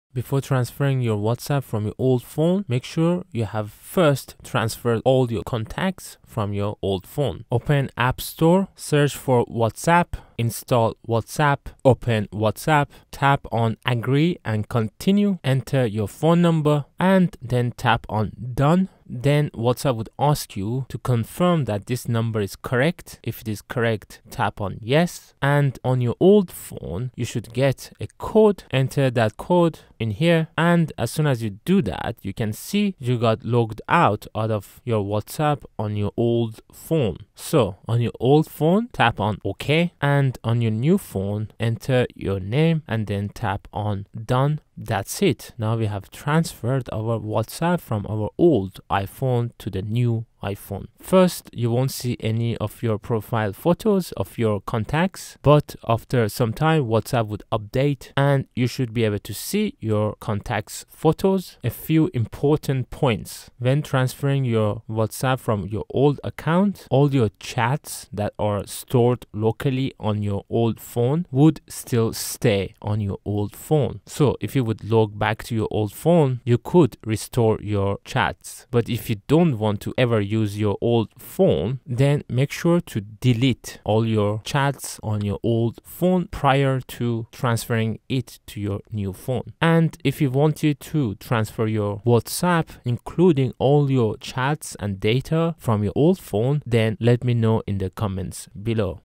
How to transfer WhatsApp from iPhone to iPhone
Have you ever wonder how easy or difficult it is to transfer your WhatsApp to your new iPhone. Watch this quick 1:30 minutes tutorial, to learn how!

Before transferring WhatsApp from your old iPhone, make sure you've transferred all your contacts first.

IMPORTANT:
If you are transferring your WhatsApp account from one phone to the other, WhatsApp won't automatically delete your chats on your old device! And, once you've moved your WhatsApp account, it won't be very easily to delete your chats stored (leftovers) on the old device.  If you don't backup your chats onto iCloud (or locally), then once you delete them from the device (phone), you won't be able to retrieve them anymore.

In the above video, I show how to transfer your WhatsApp when you don't backup your chats. If you backup your chats, then you could transfer your chats from your old phone to your new iPhone as well.

I have put a lot of efforts in making this video! It does really help.